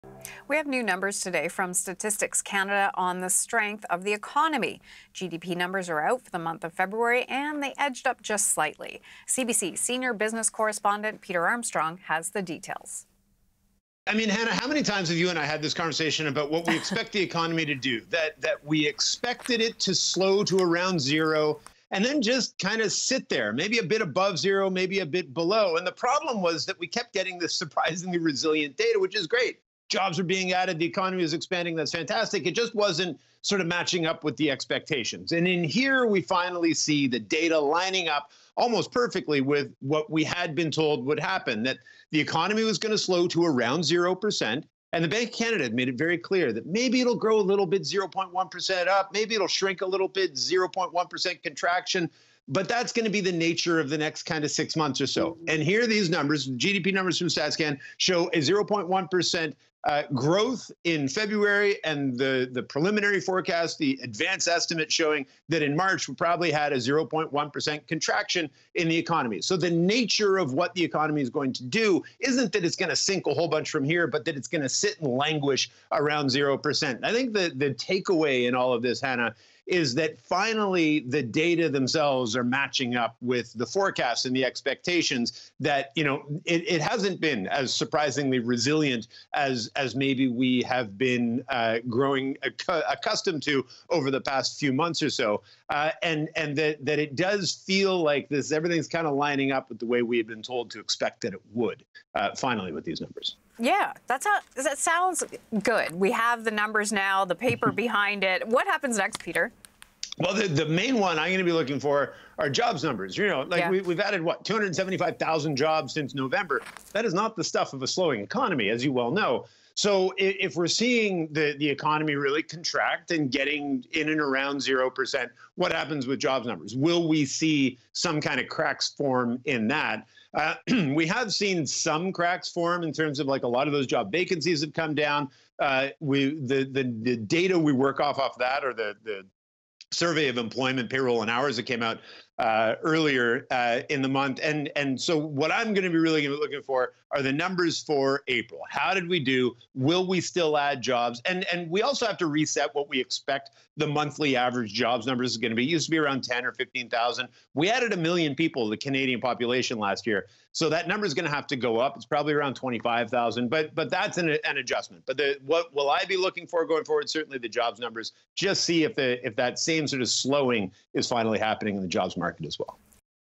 Statistics Canada says GDP increased 0.1 per cent in February
The Canadian economy grew by 0.1 per cent in February, Statistics Canada said Friday. In its latest report on economic growth, the federal agency's preliminary estimate suggested real gross domestic product grew at an annualized rate of 2.5 per cent in the first quarter.